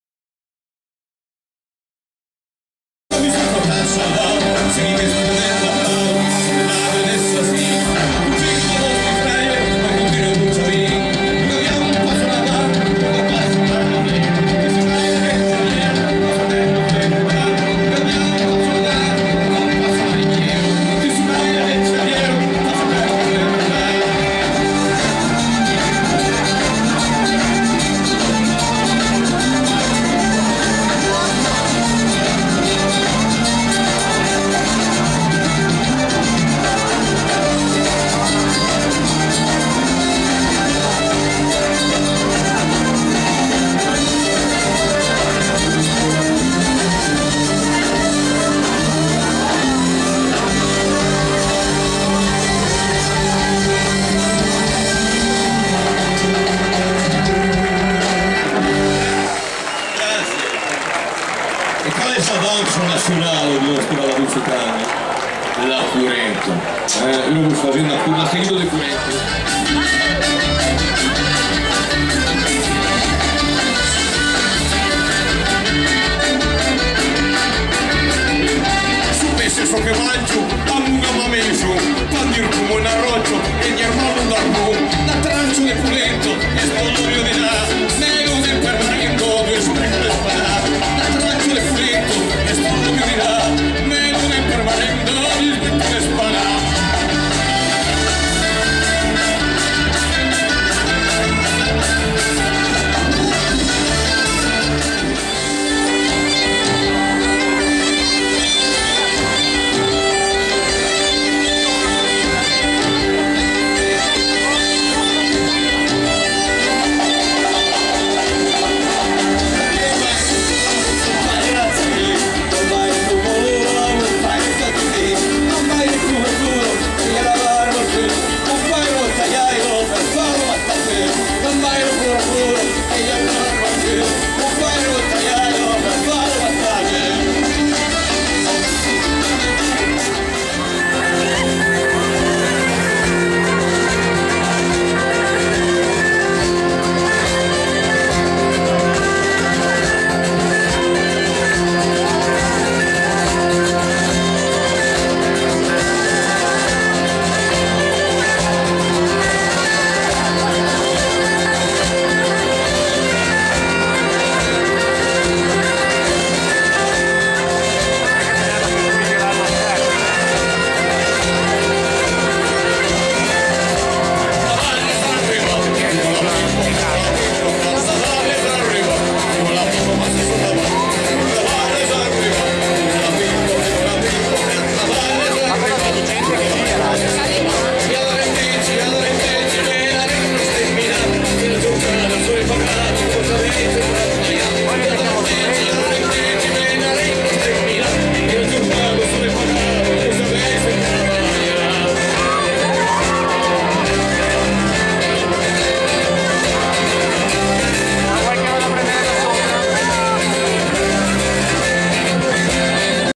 Grande concerto occitano a Pomaretto
La Peiro Douso e Lou Dalfin in concerto a Pomaretto (TO) il 31 luglio 2011. Grande spettacolo, grande pubblico e sportello itinerante presente!